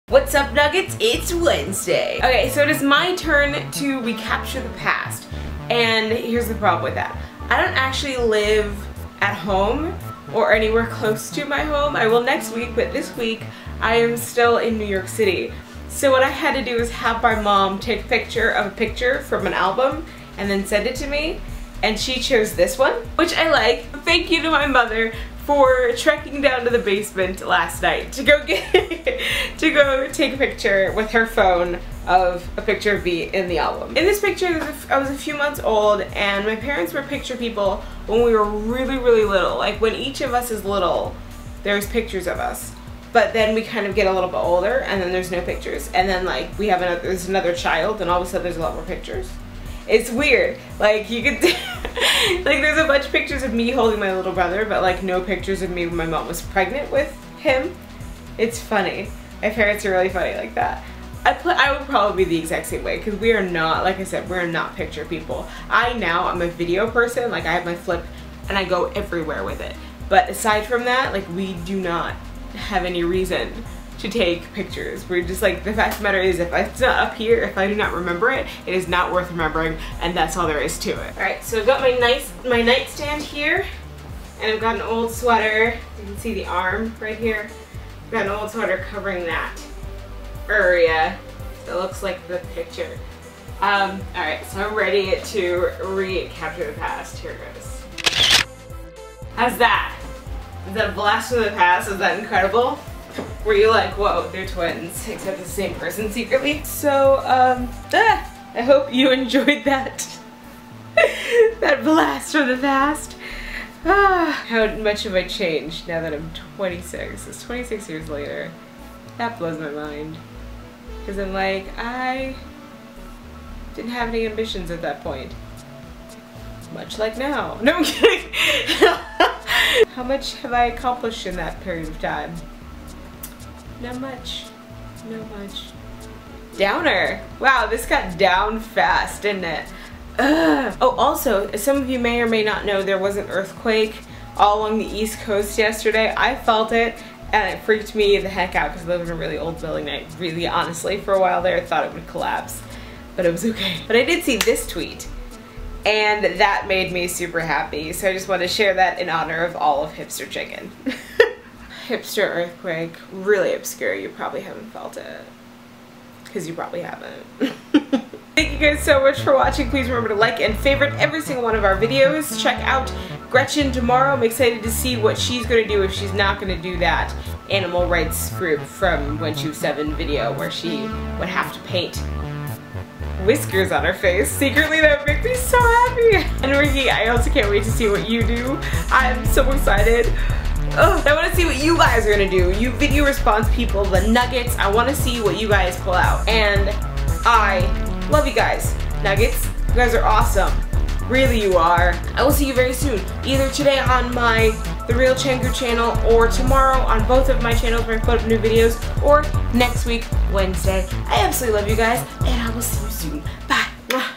Wed. Hipster Earthquake!?!?!
In which Chengu shares how she got the picture needed for this week and how she has/hasn't changed since the picture was taken.

To see the vids I was talking about,